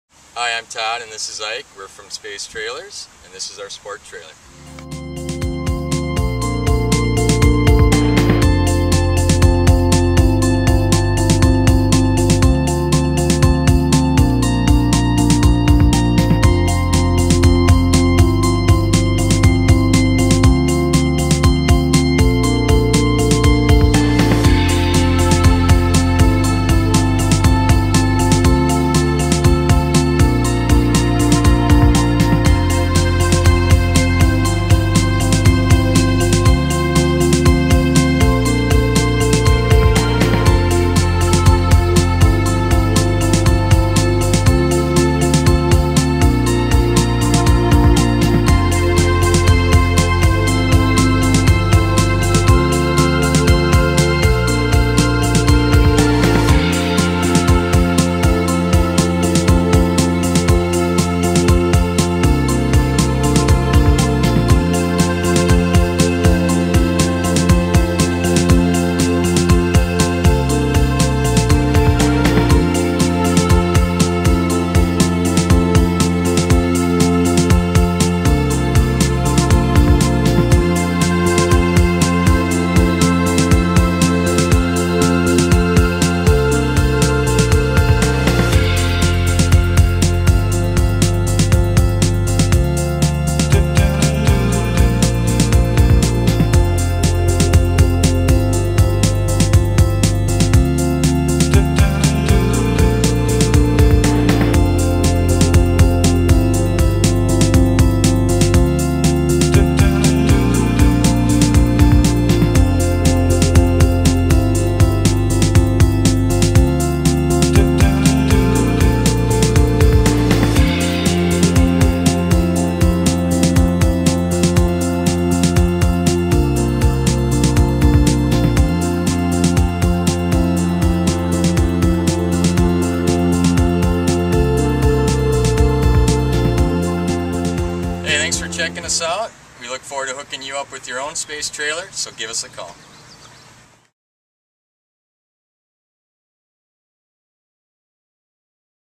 Lightweight Enclosed Sport Trailers - SPACE Trailer
Lightweight enclosed sport trailers for hauling kayaks, bikes and gear.  Made in the USA and delivered to your driveway.